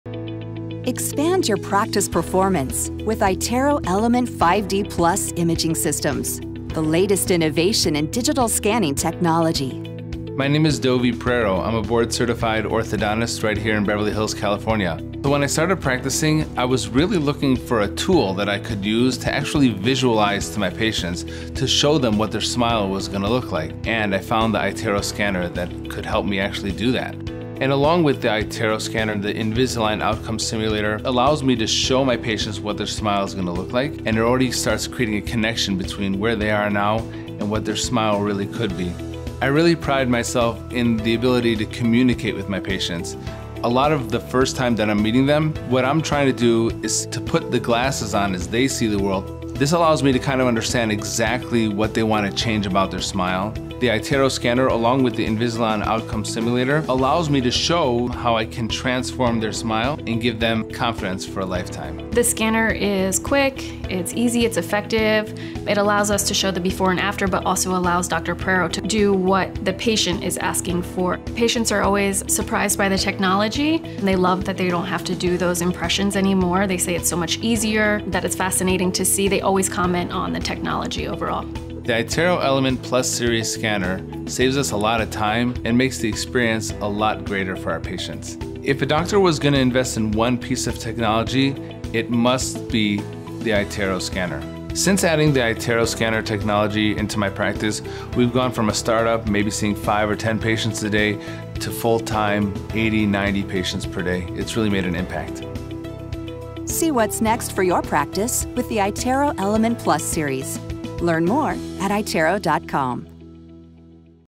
Boost practice performance with the iTero Element™ 5D Plus imaging system
Looking to enhance your practice performance? Discover the iTero Element™ 5D Plus imaging system, one of the latest innovations in digital scanning technology. Dr. Dovi Prero, a board-certified orthodontist from Beverly Hills, California, showcases how the iTero Element™ 5D Plus can transform your practice. This cutting-edge system not only enhances patient communication by showing potential smile outcomes but also increases daily patient volume dramatically.

With the iTero Element™ 5D Plus imaging system, expect:
- Digital scanning technology for precise orthodontic assessments
- The Invisalign® Outcome Simulator to visualize treatment outcomes
- iTero Multi-Direct Capture™ (MDC) technology for superior imaging
- A lifelike 3D model and integrated 3D intraoral camera for detailed analysis

Dr. Prero's experience highlights the scanner’s effectiveness in boosting practice efficiency, handling up to 90 patients per day. Learn how iTero™ scanner can revolutionize your practice

*Data on file at Align Technology, as of August 8, 2024.